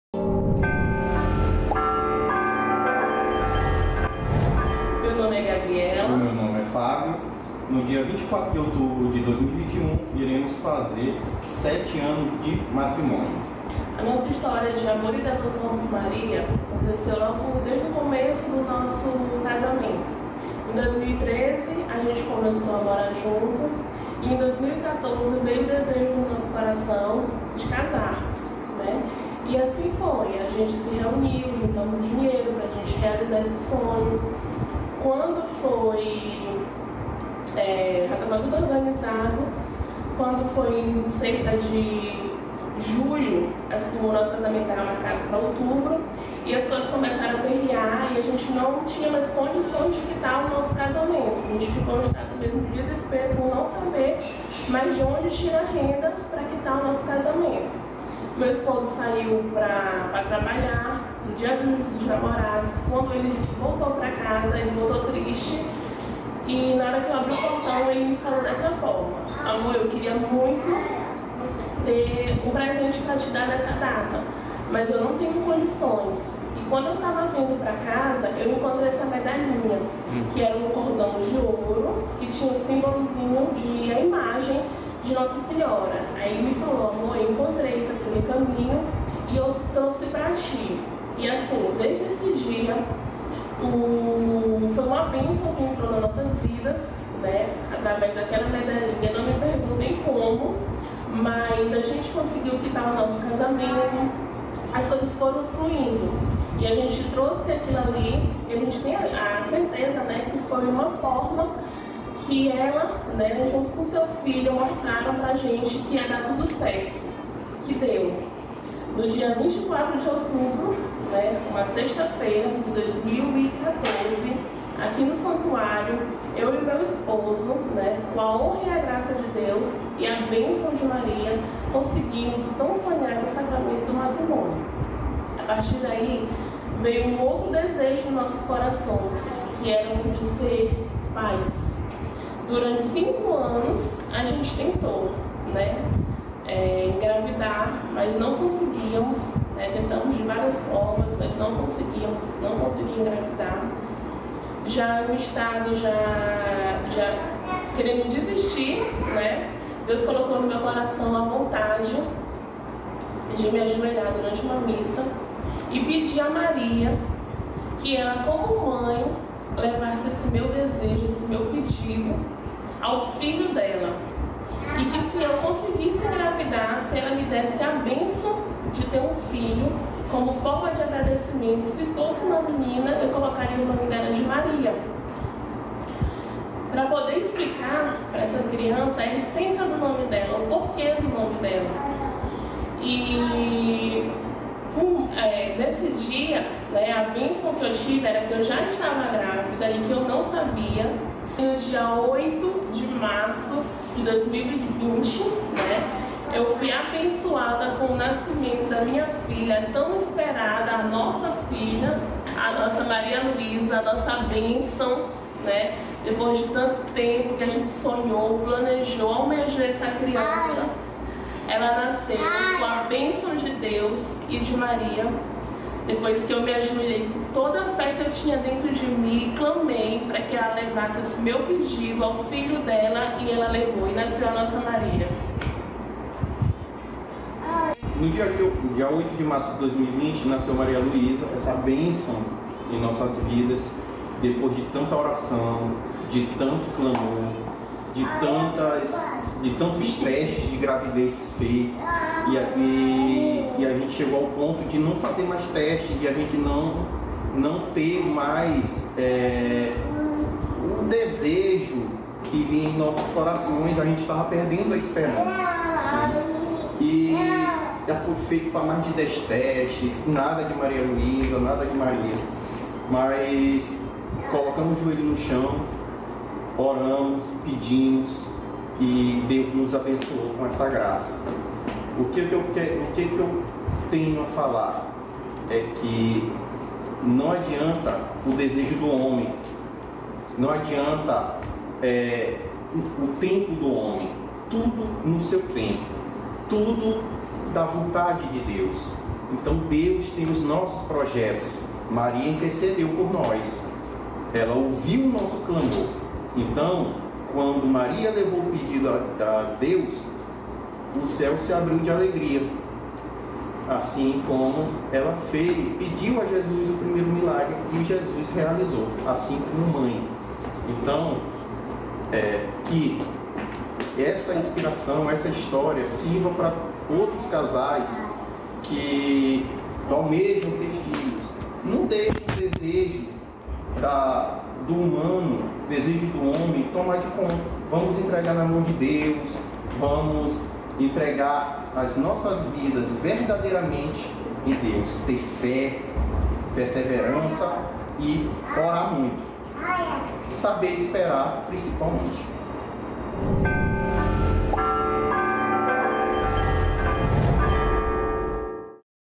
HISTÓRIA DE DEVOÇÃO
Vamos conhecer a história de devoção a Nossa Senhora dos nossos irmãos Fabio e Gabriela